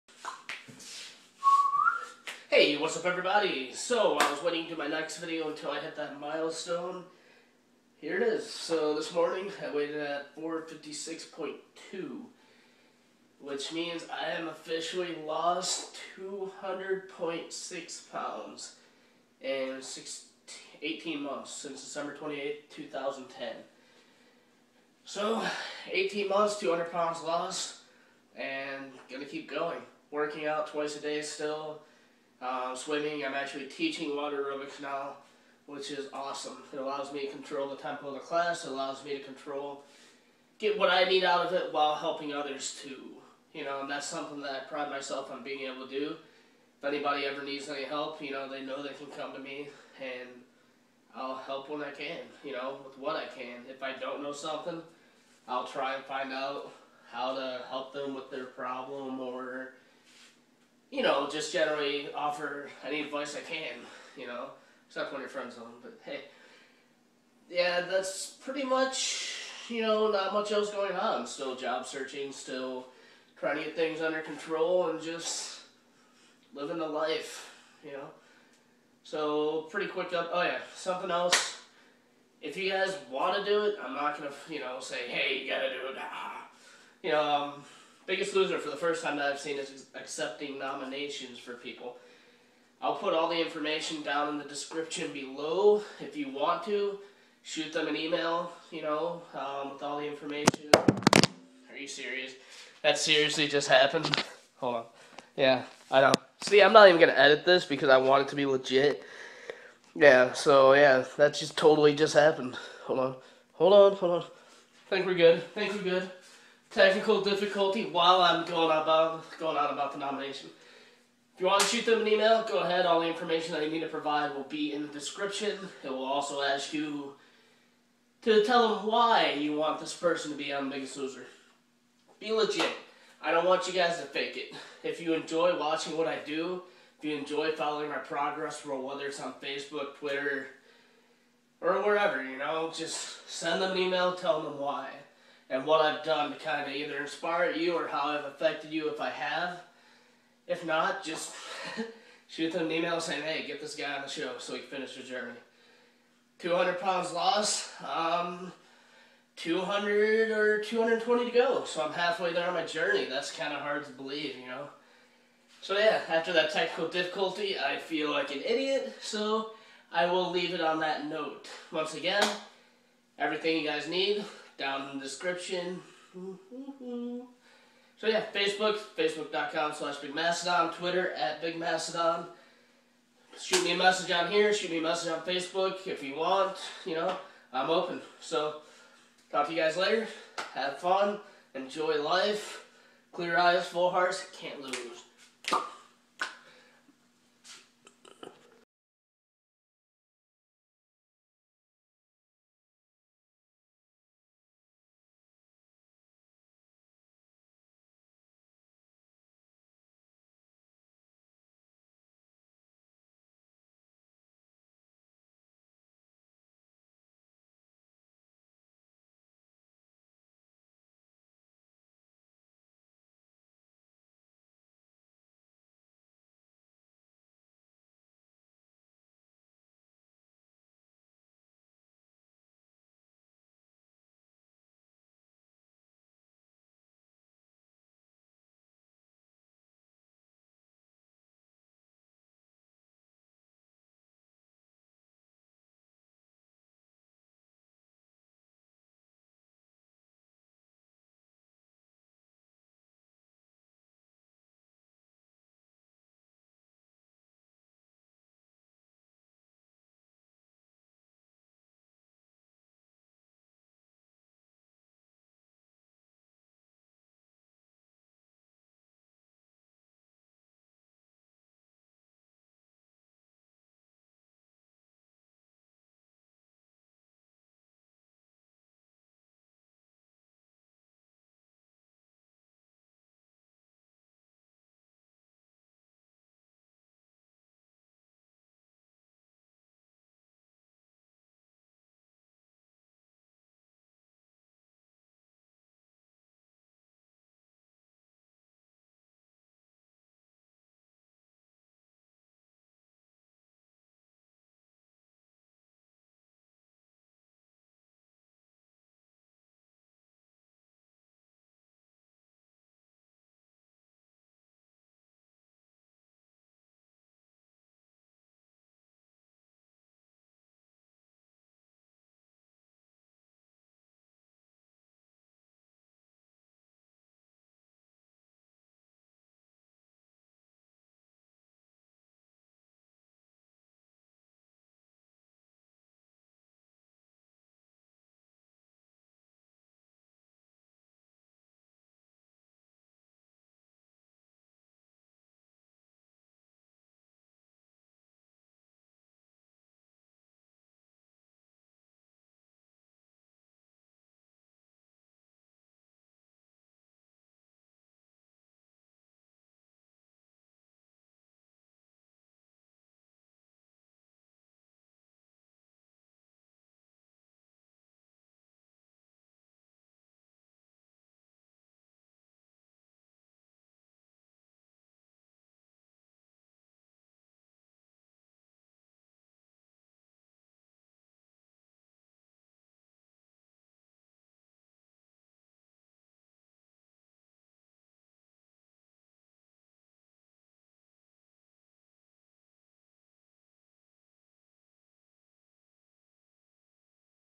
Milestone achieved.. and Biggest Loser stuff..
EDIT:  Apparently this video decided to upload twice.. even though it shouldn't have.  It's only like three minutes and 10 seconds long.. so ignore the second half of it, lol.  This seemed to be a technical difficulty plagued video with the camera falling and now this.  So yeah.. I fail.  I don't feel like re-uploading it since it's already up.

Hey all.. this video is basically me announcing my 200 pound loss milestone.  18 month process but I finally got there.  Almost half of my journey down.. another half is beginning.  It's definitely not easy but I will get there.

The Biggest Loser Nomination information.. this is something I haven't seen them do before so I'm kinda trying to rally the troops (so to speak) to let them know that I would be a good candidate for the show and people want to see me finish my journey on there.  You by no means have to do this.. but I would greatly appreciate it if you took the time to send in an email about me.  Here is the information:

"Can I nominate someone who I think would be great for the show?
Yes! Please send nominations to . Please include your name, age and place you reside and the nominee's name, age, current city/state and why they deserve the chance to be on The Biggest Loser."

Please put legit information as I do not want to rub people the wrong way by having false information sent in.  My information is:  Justin Heard/25/Plano, IL and your reasoning for me being on the show.

Thank you for your time and I appreciate the support you all have given me throughout the years.

-  I had to keep the camera falling bit in there as that was way too perfect...